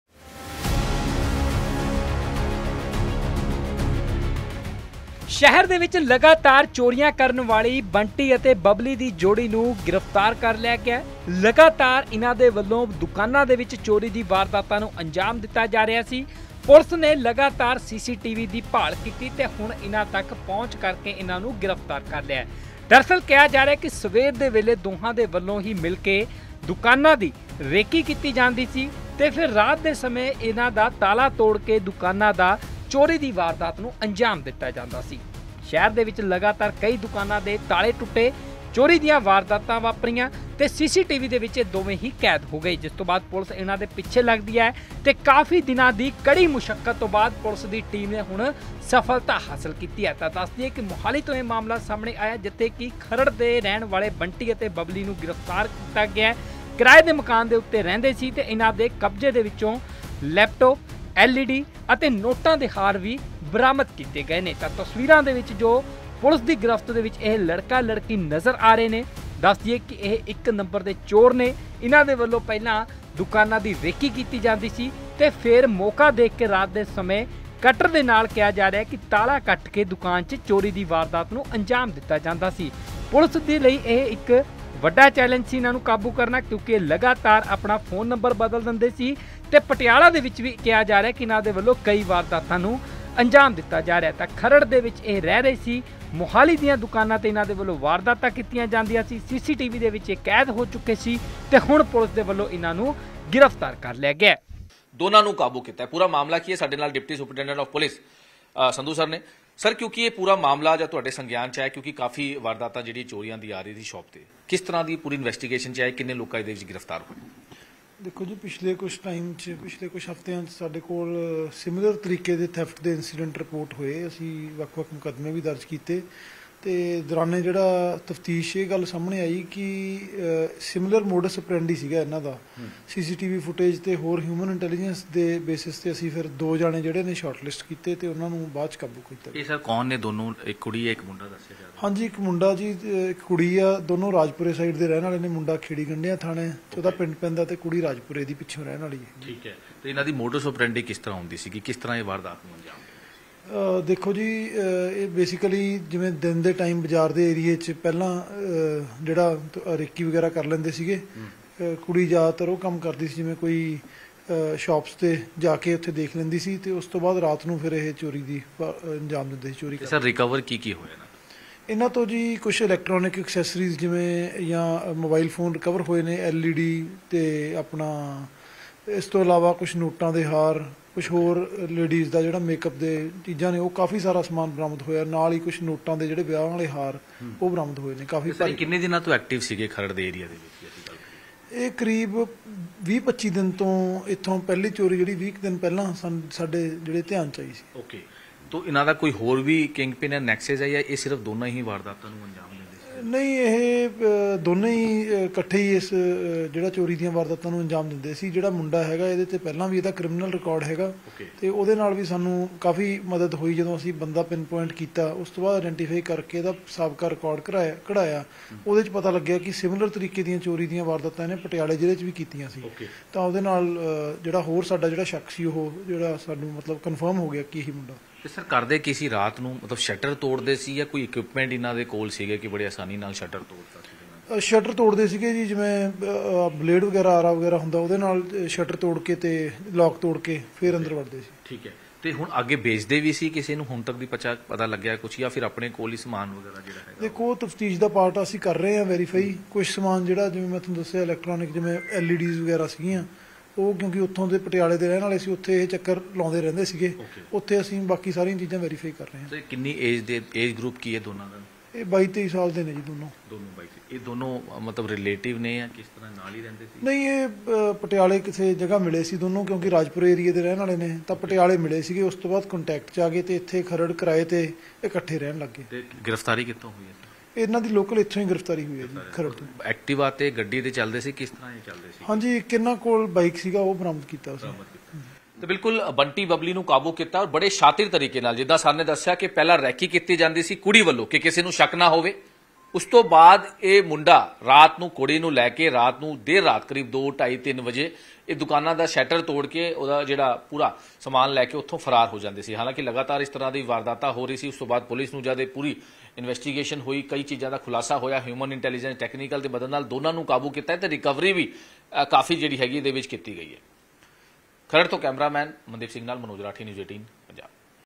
Mohali ’ਚ Bunty bubbly ਦੀ ਜੋੜੀ ਪੁਲਿਸ ਦੀ ਗ੍ਰਿਫ਼ਤ ’ਚ... ਦਿਨ ’ਚ ਰੇਕੀ ਤੇ ਰਾਤ ਨੂੰ ਵਾਰਦਾਤ
ਬੰਟੀ-ਬੱਬਲੀ ਦੀ ਜੋੜੀ ਪੁਲਿਸ ਦੀ ਗ੍ਰਿਫ਼ਤ ’ਚ... ਦਿਨ ’ਚ ਰੇਕੀ ਤੇ ਰਾਤ ਨੂੰ ਵਾਰਦਾਤ